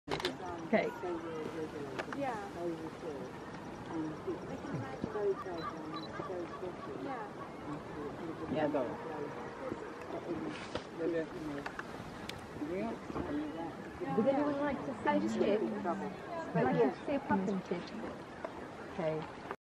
Watch this adorable newborn Puffin Chick on Skomer Island...
The adorable newborn Puffin Chick trying to get a glimpse of the outside world, while protective Mamma Puffin is shooing it back into the safety of the burrow, shielding it from the vicious Seagulls ready to attack!!